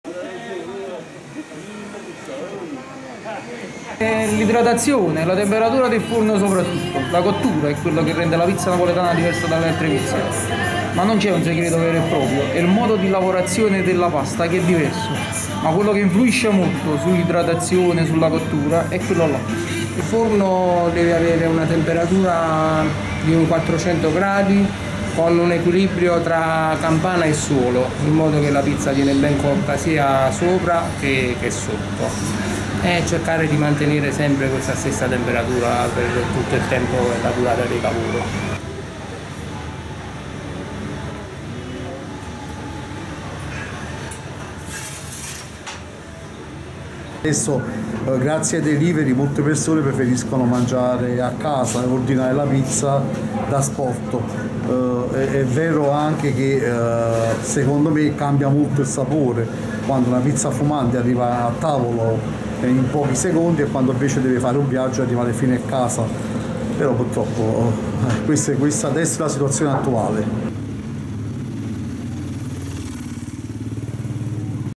I segreti della pizza napoletana svelati dai pizzaioli del centro di Napoli
(Agenzia Vista) - Roma, 17 Gennaio 2022 - Forno e manualità, questi sono i segreti della pizza napoletana secondo i pizzaioli del centro storico di Napoli. Ma attenzione, con l'asporto il sapore cambia. Le interviste realizzate in occasione della Giornata Internazionale della Pizza.